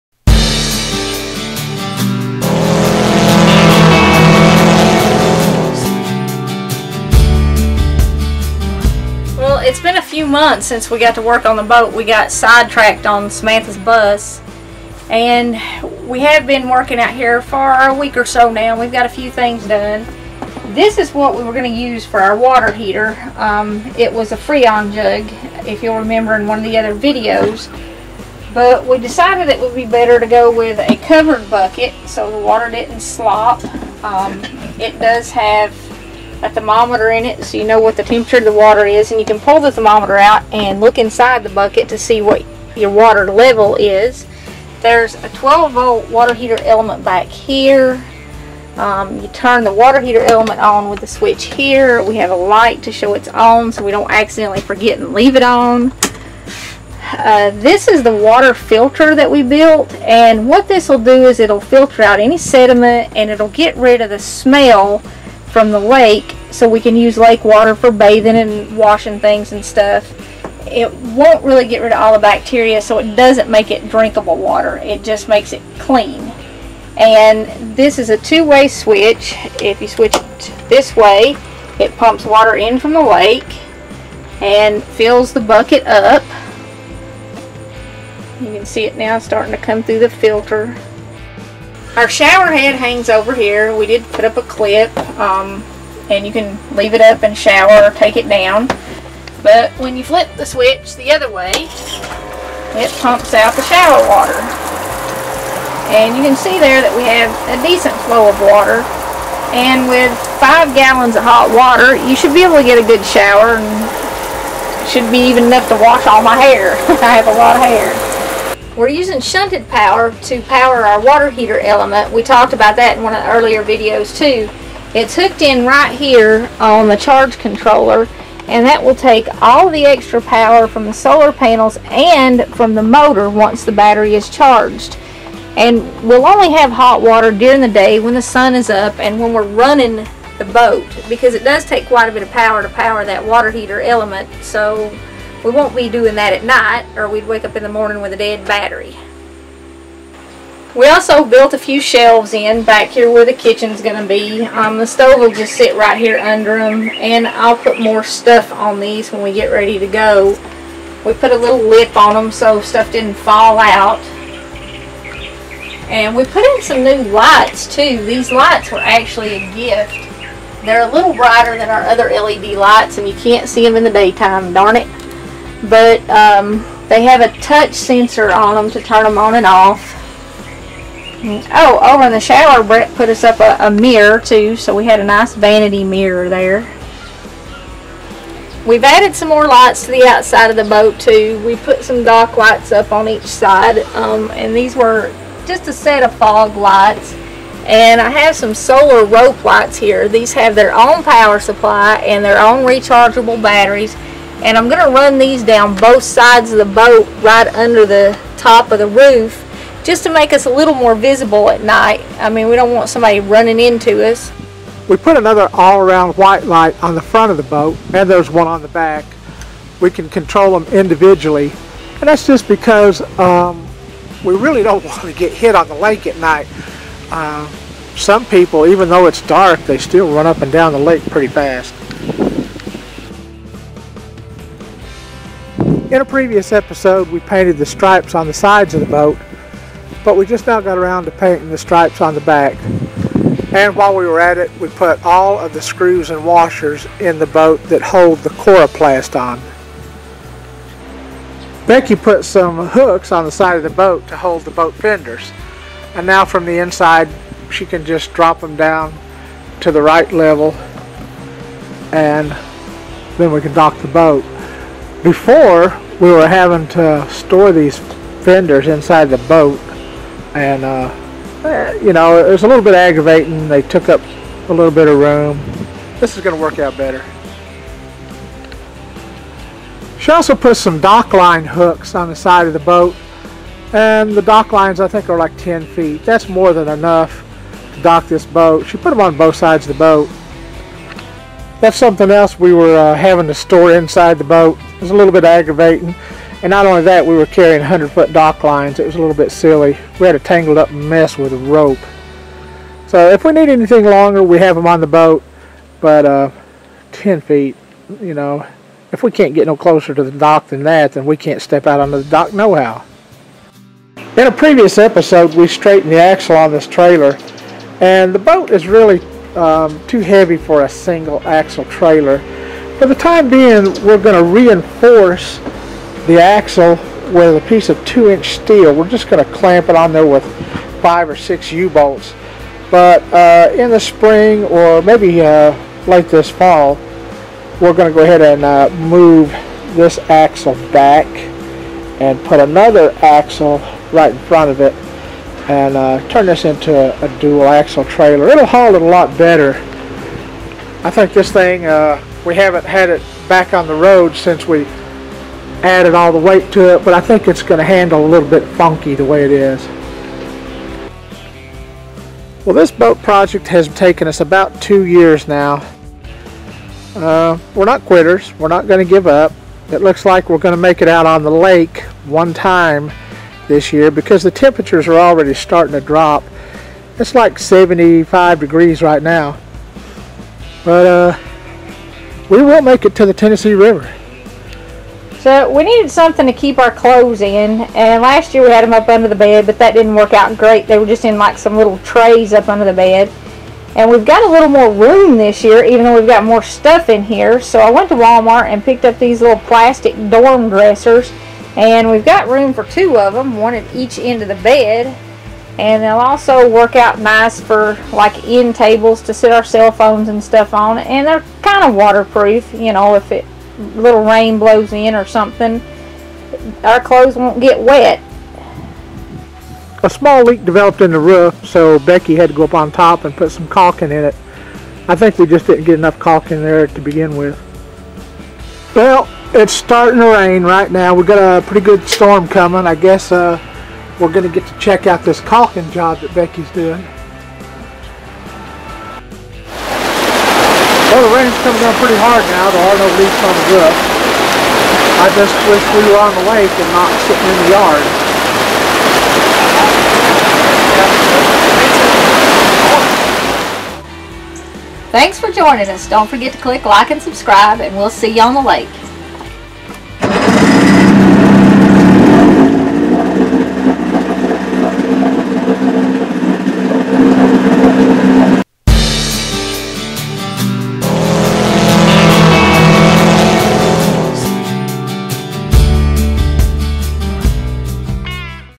Ep.16 - Resuming Work On The Boat
We had to halt work on the boat and help our daughter with one of her projects; "This Old Bus."  We finally resumed work on the boat but time is running out if we're going to get it in the water this year. Temperatures are falling and winter will be here before we know it.